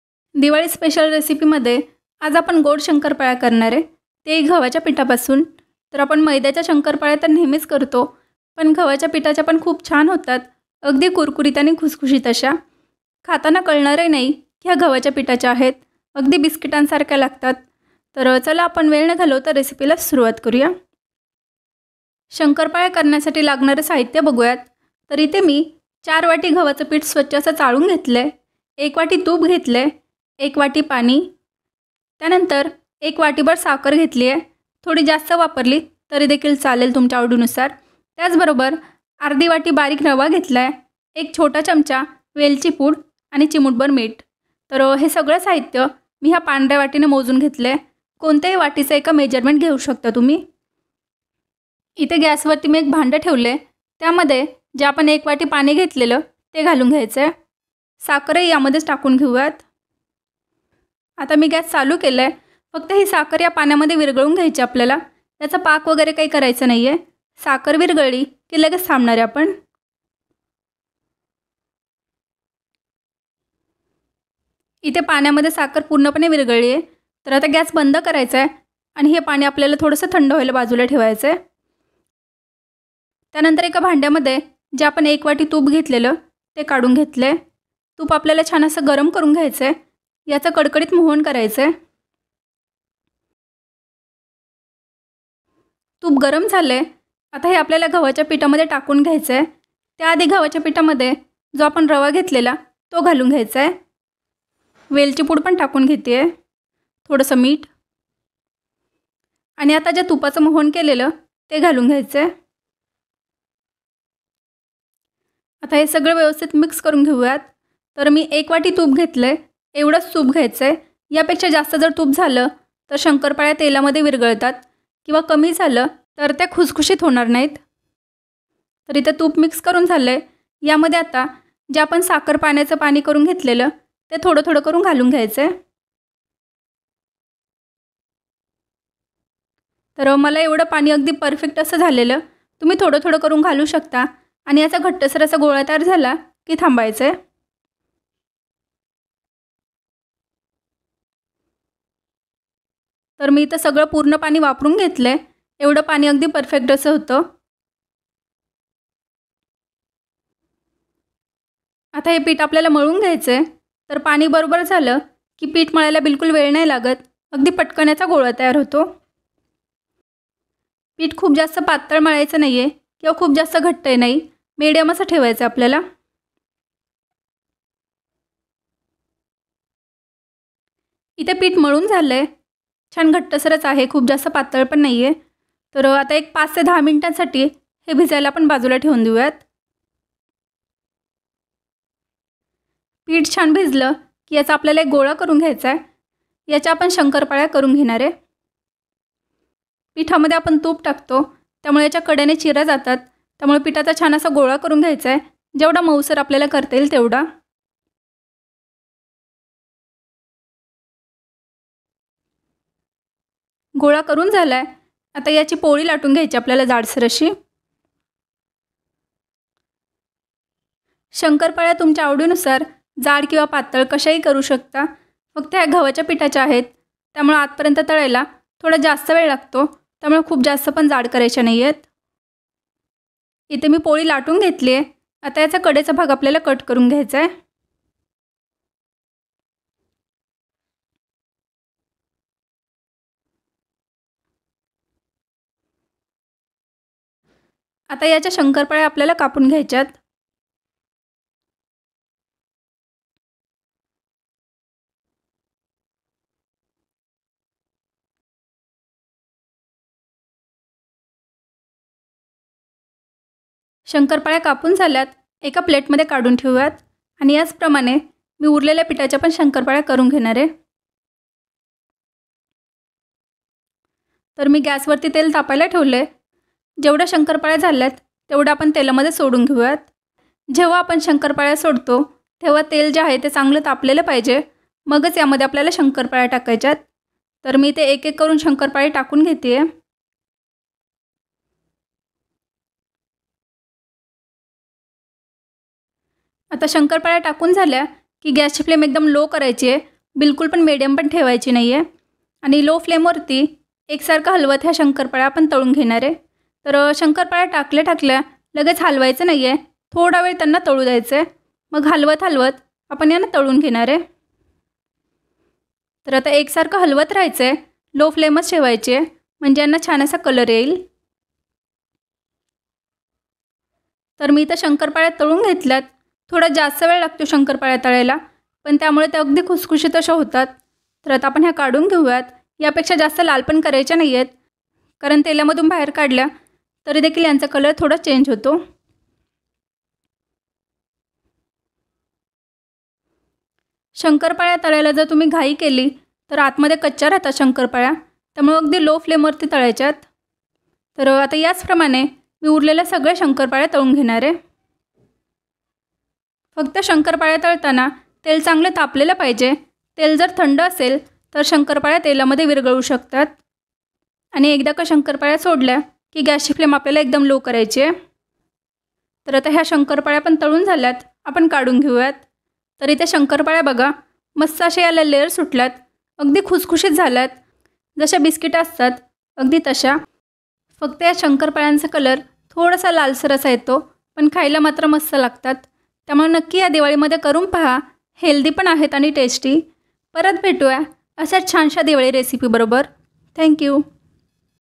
शंकरपाळी|गव्हाच्या पिठाची बिस्किटं सारखी खुसखुशीत आणि लेयेर्स असलेली शंकरपाळी|Diwali recipe
साहित्य
४ वाट्या गव्हाचं पीठ
१ वाटी तूप
१ वाटी पाणी
१/२ वाटी बारीक रवा
१ चमचा वेलची पूड
तळण्यासाठी तेल

Ingredient
4 cup wheat flour
1 cup ghee
1 cup water
1/2 cup fine rava
1 tsp cardamom powder